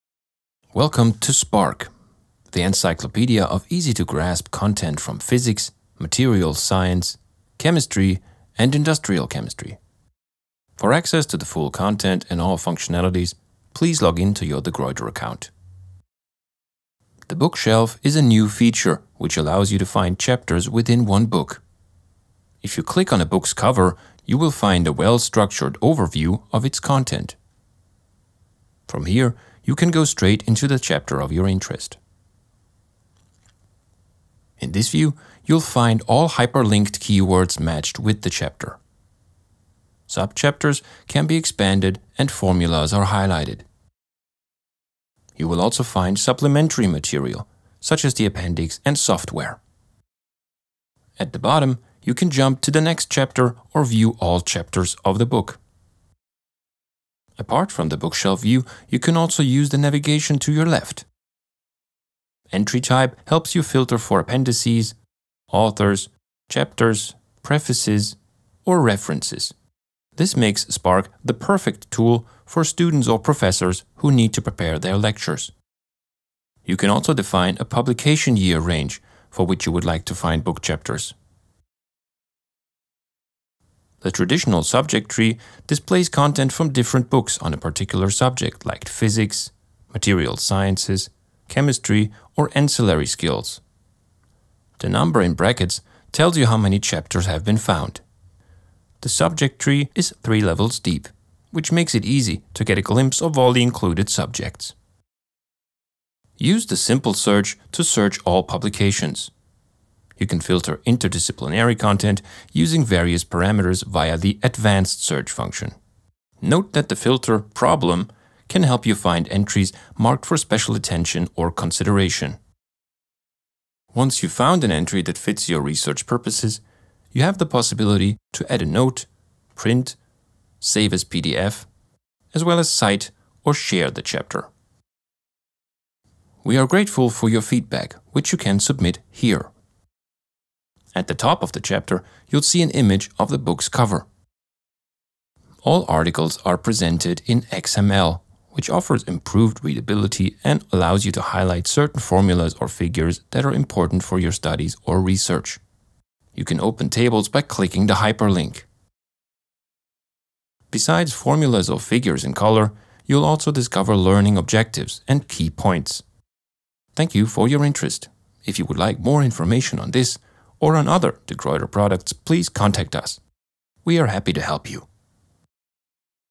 Tutorial: Spark Online Reference
Spark offers up-to-date, interdisciplinary, and cross-linked introductions into a multiplicity of subjects in natural sciences. As the first dynamical Online Reference it contains a comprehensive compilation of more than 1.000 Articles, frequently updated and extended by over 50 authors.

Young academics and postgraduates will find the all the essential material, to gain basic expertise on all subject areas of cutting edge research.